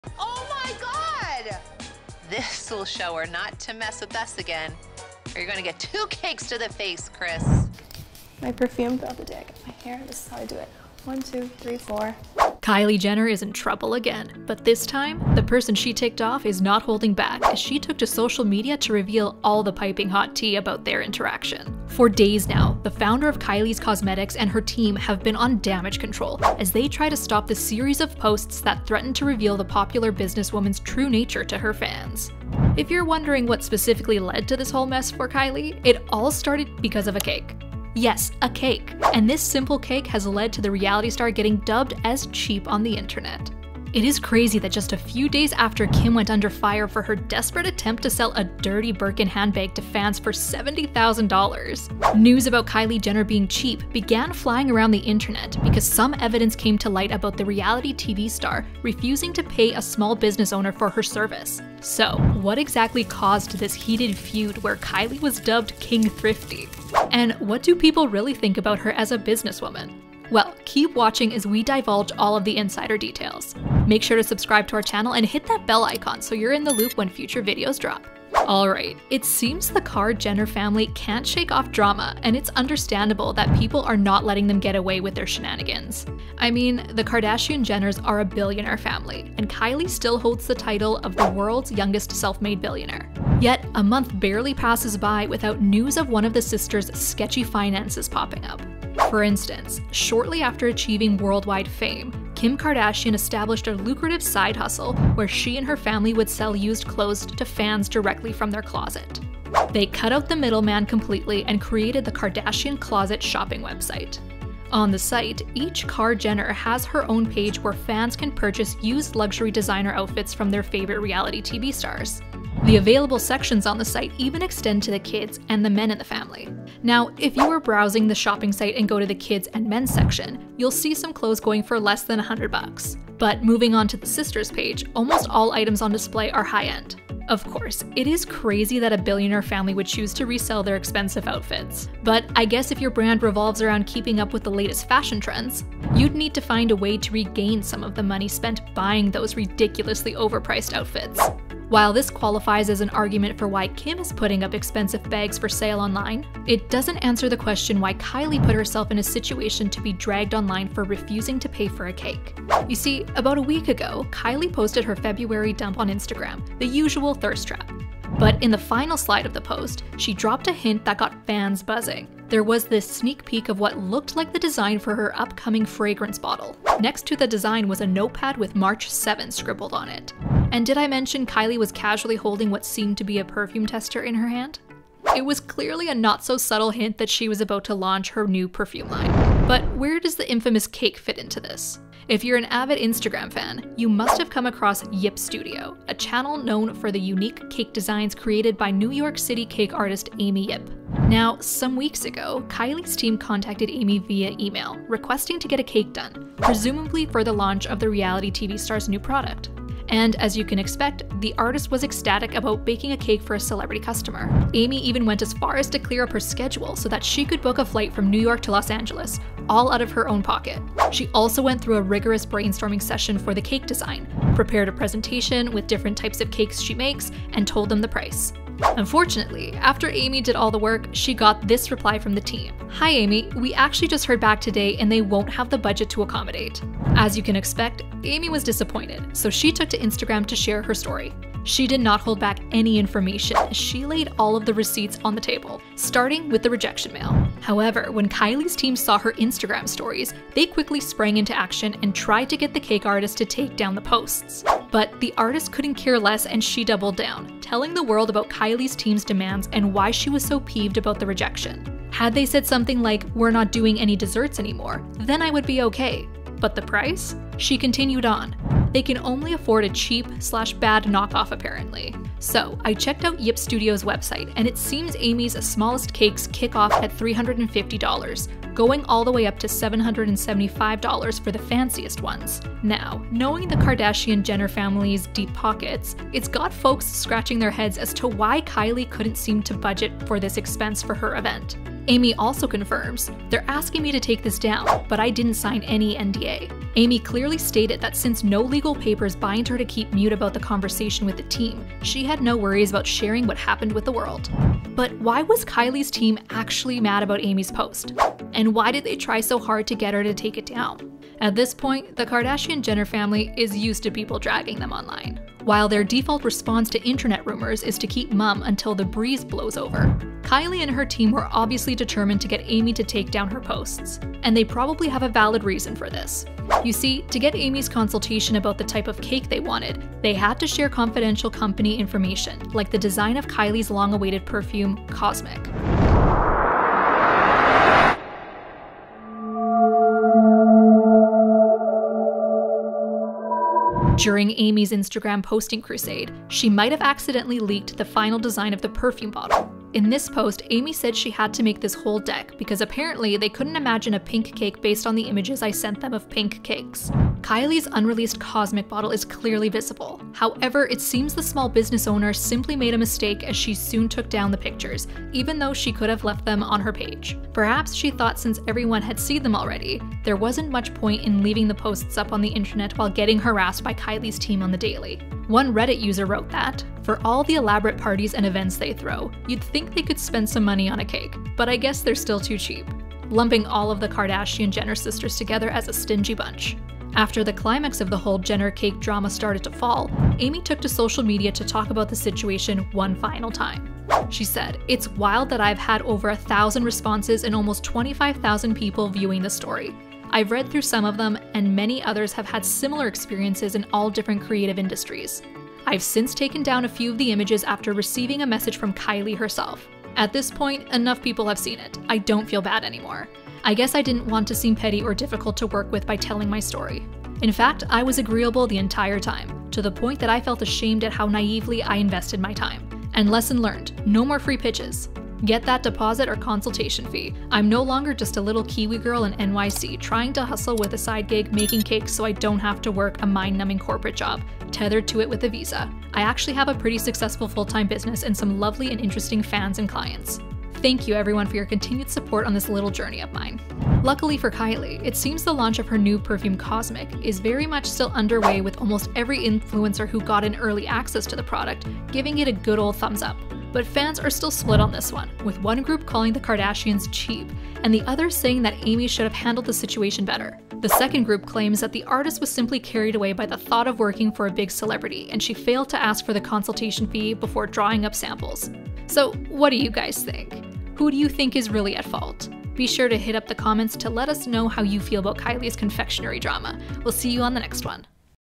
Kylie Jenner SLAMMED For Being Super CHEAP (She rips off Small Businesses??)
Kylie Jenner is in trouble again, but this time, the person she ticked off is not holding back as she took to social media to reveal all the pipping hot tea about their interaction.

For days now, the founder of Kylie’s Cosmetics and her team have been on damage control as they try to stop the series of posts that threatened to reveal the popular businesswoman’s true nature to fans.

If you are wondering what specifically led to this whole mess for Jenner, it all started because of a cake. Yes, a cake! And this simple cake has led to the reality star getting dubbed as “cheap” on the internet.

It is crazy that just a few days after Kim went under fire for her desperate attempt to sell a “Dirty” Birkin handbag to fans for 70 thousand dollars, news about Kylie Jenner being “cheap” began flying around the internet because some evidence came to light about the reality TV star refusing to pay a small business owner for her service. So, what exactly caused this heated feud where Kylie was dubbed King Thrifty? And what do people really think about her as a business woman? Well keep watching as we divulge all the insider details